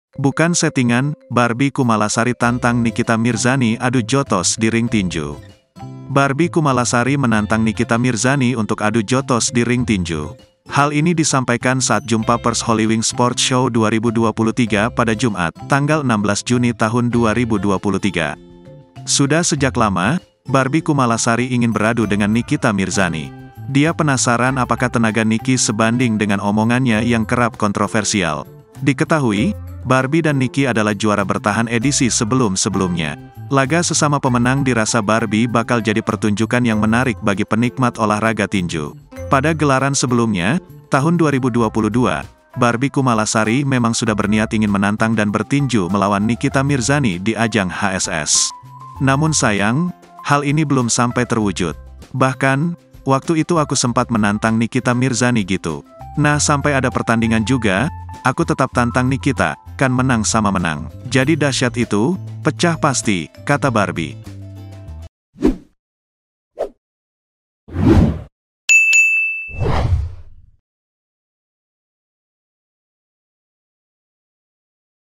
'Bukan Settingan' Barbie Kumalasari Tantang Nikita Mirzani Adu Jotos di Ring Tinju
- Barbie Kumalasari menantang Nikita Mirzani untuk adu jotos di ring tinju.

Hal ini disampaikan saat jumpa pers Holywing Sport Show 2023 pada Jumat (16/6/2023).

Sudah sejak lama, Barbie Kumalasari ingin beradu dengan Nikita Mirzani.